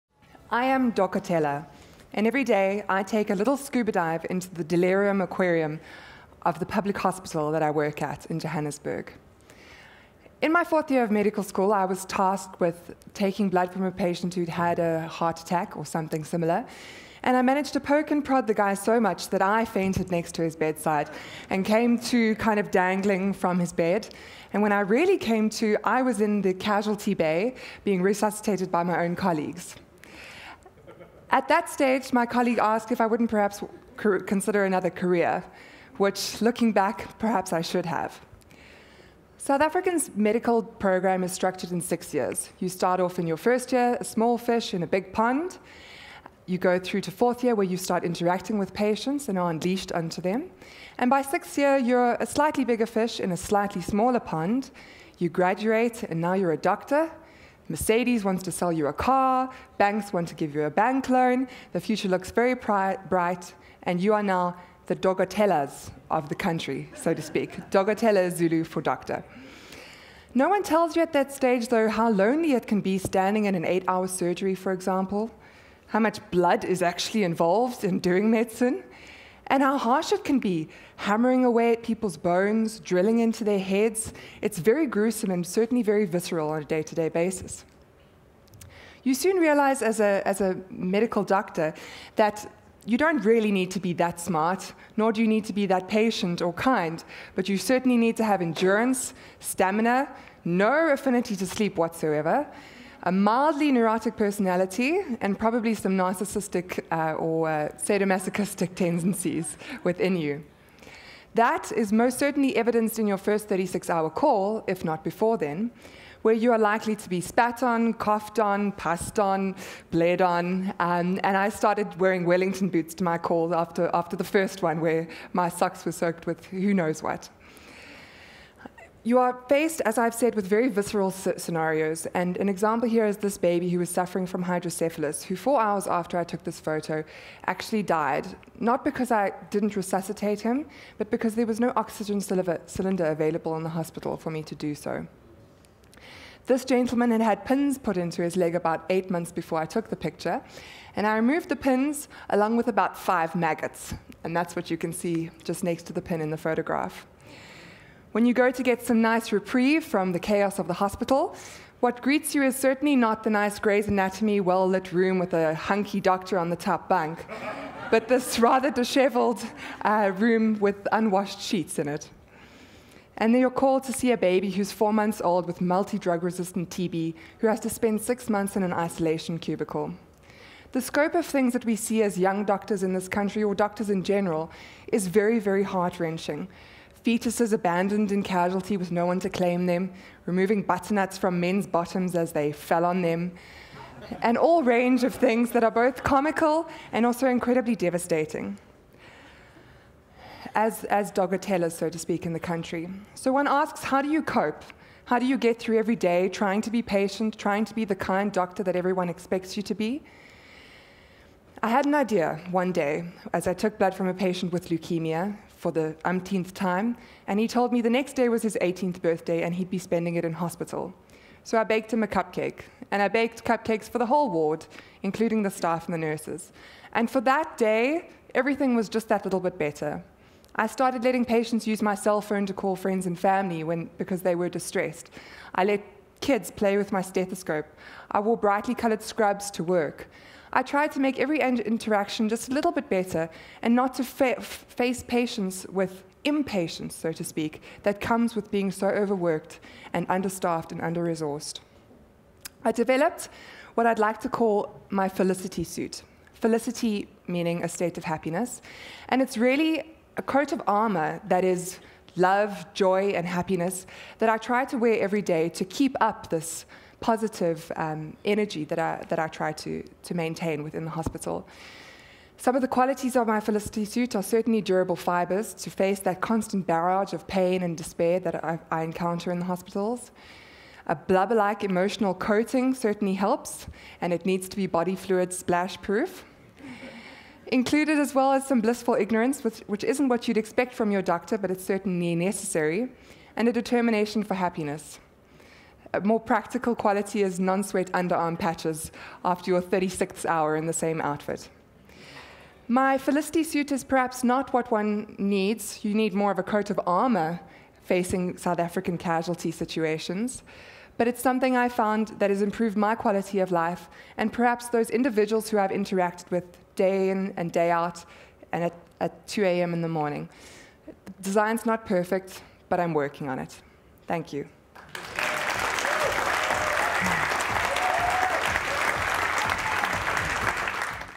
Robyn Grobler: Why I need a felicity suit
Robyn Grobler, a dokotela (Zulu for "doctor") and hero to the wounded passing through her medical clinic, believes that smiling helps curb patients' pain.